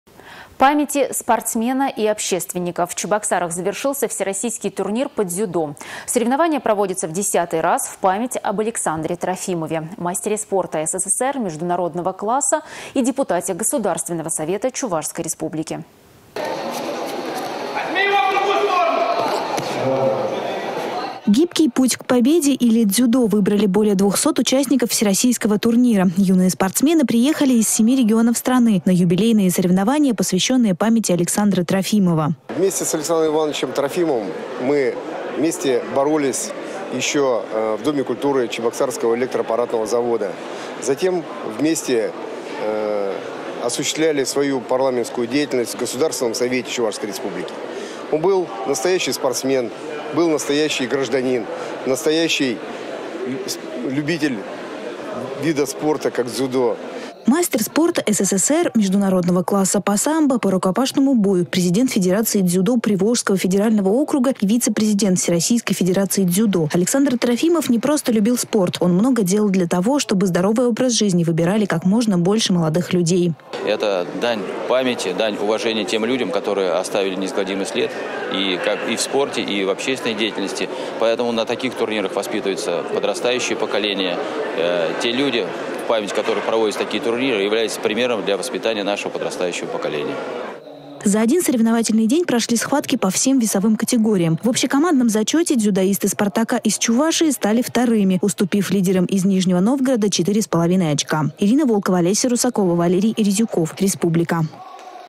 В Чебоксарах завершился всероссийский турнир по дзюдо.
Соревнования проводятся в 10-й раз в память об Александре Трофимове -  мастере спорта СССР международного класса и депутате Государственного Cовета Чувашской Республики.